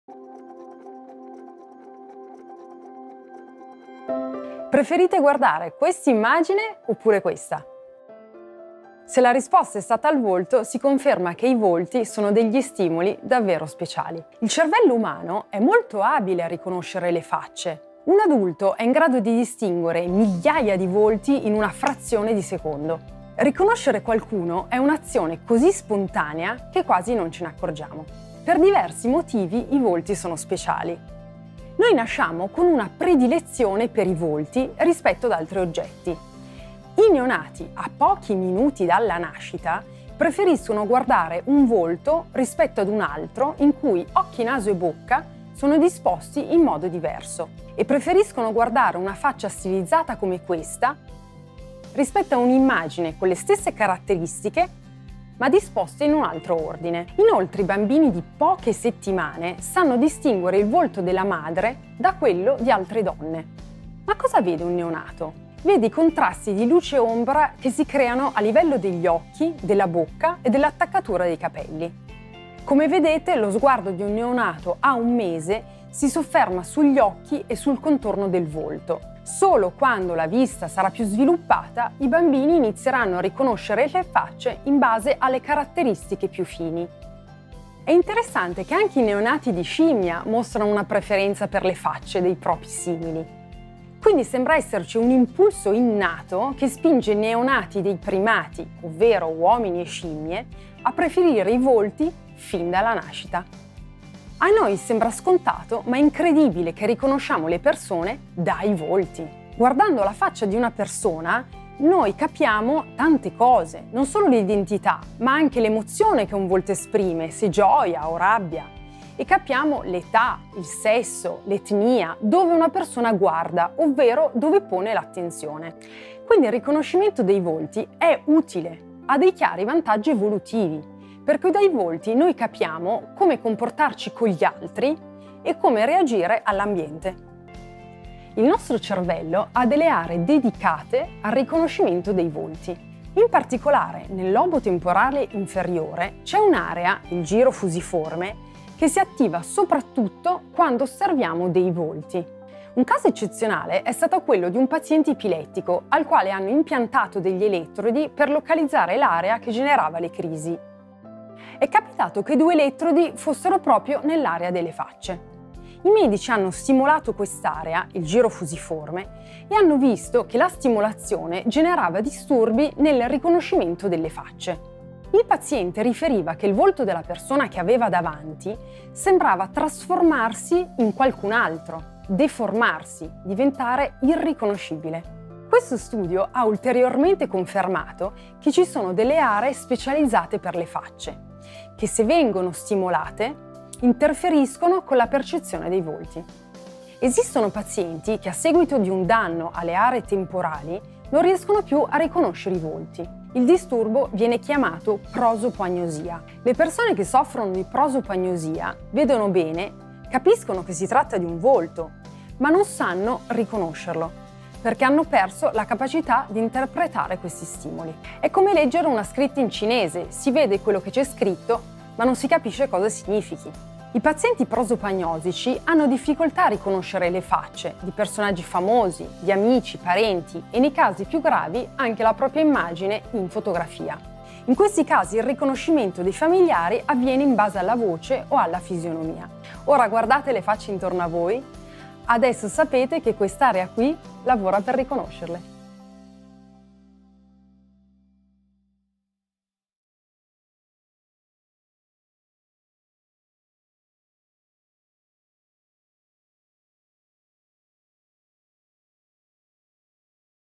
Come il cervello riconosce le facce? Pillola #3
I volti sono una categoria di stimoli davvero speciale per diversi motivi:
1. Noi nasciamo con una predilezione per i volti rispetto ad altri oggetti.
I neonati preferiscono guardare un volto rispetto ad un’altra immagine, anche molto simile.

2. Noi riconosciamo le persone guardando i volti. Dai volti noi capiamo l’età, il sesso, l’etnia e dove una persona guarda. Le facce sono utili da un punto di vista evolutivo perché guardando un volto capiamo come comportarci con gli altri e nell’ambiente.

3. Il cervello ha delle aree che si sono specializzate per il riconoscimento dei volti. In particolare nel lobo temporale c’è un’area chiamata giro fusiforme che si attiva soprattutto quando osserviamo i volti.

4. Esistono pazienti che, a seguito di un danno alle aree temporali, non riescono più a riconoscere i volti. Il disturbo si chiama prosopoagnosia.

Referenze:

- Chien SH. No more top-heavy bias: infants and adults prefer upright faces but not top-heavy geometric or face-like patterns. J Vis. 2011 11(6).
- Kanwisher N, Yovel G. The fusiform face area: a cortical region specialized for the perception of faces. Philos Trans R Soc Lond B Biol Sci. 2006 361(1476):2109-28.
- Maurer D, Barrera M. Infants' perception of natural and distorted arrangements of a schematic face. Child Dev. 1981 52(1):196-202.
- Pancaroglu R, Hills CS, Sekunova A, Viswanathan J, Duchaine B, Barton JJ. Seeing the eyes in acquired prosopagnosia. Cortex. 2016; 81:251-65.
- Parvizi J, Jacques C, Foster BL, Witthoft N, Rangarajan V, Weiner KS, Grill-Spector K. Electrical stimulation of human fusiform face-selective regions distorts face perception. J Neurosci. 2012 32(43):14915-20.
- Sugita Y. Face perception in monkeys reared with no exposure to faces. PNAS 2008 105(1):394-8.
- Test di riconoscimento di volti di personaggi famosi -versione di Bizzozero et al., 2007.